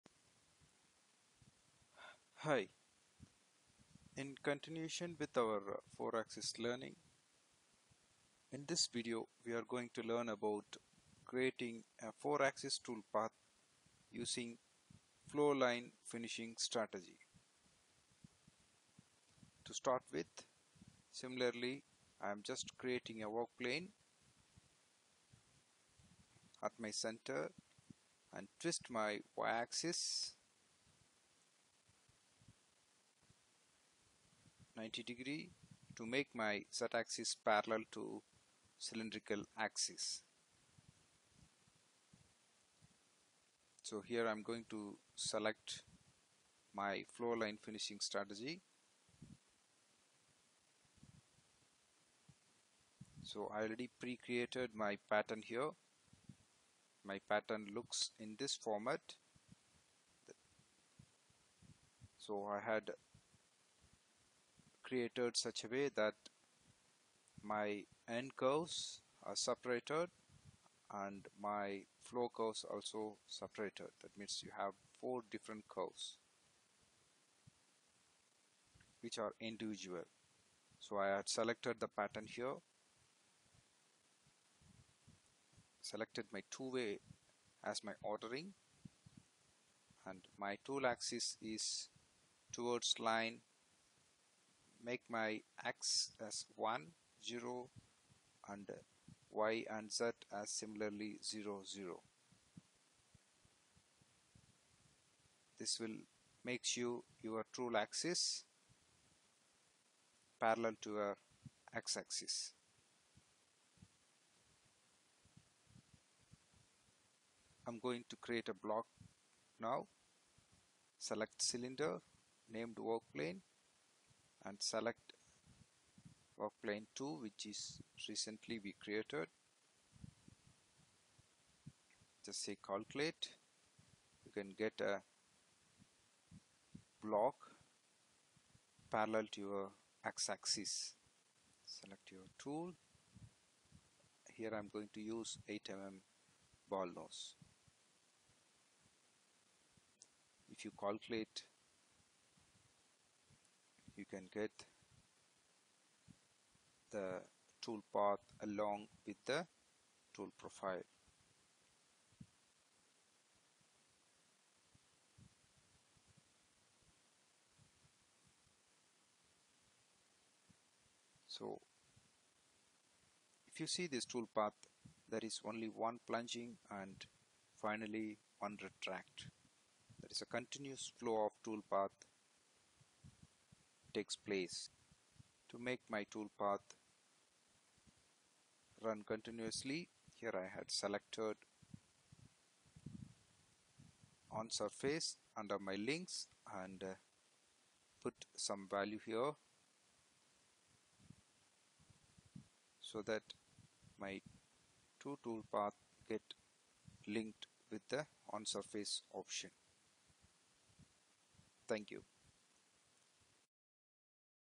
Autodesk PowerMill - 4 Axis using Flowline Finishing Strategy
Creating 4 axis toolpath strategy using Flowline finishing.